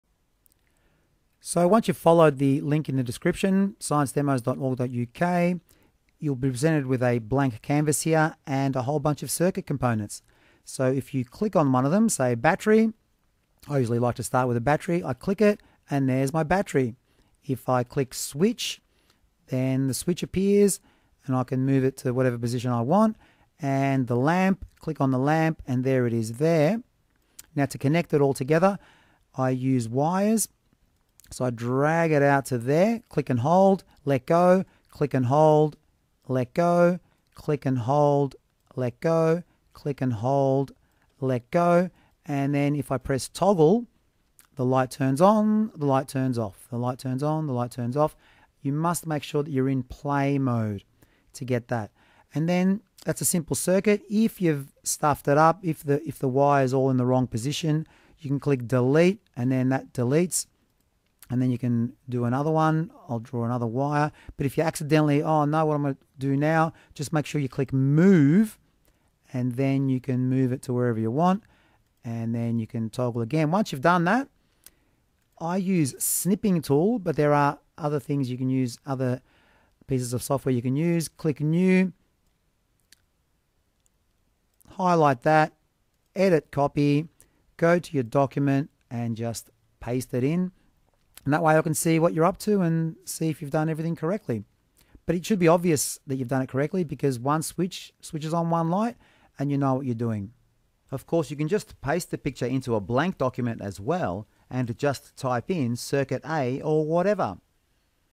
Electric Circuits Simulation Software (with Student Activity Sheets)
This excellent and completely free online resource can be used to complete the electric circuits practical activities